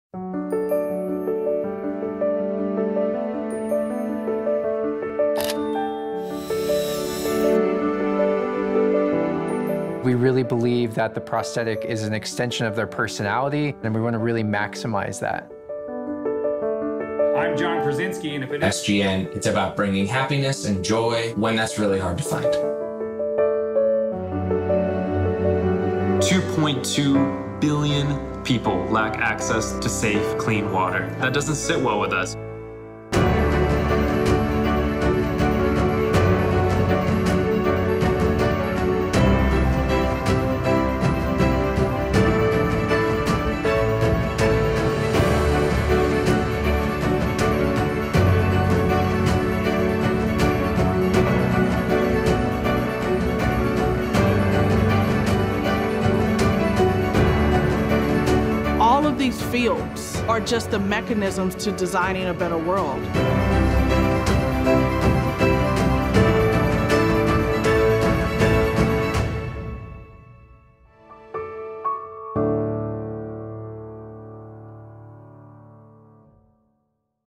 Adobe Celebrates Changemakers
Creativity can help transform lives, lift up communities, and build a more sustainable future. Discover Adobe customers who use creativity to address important social and environmental causes.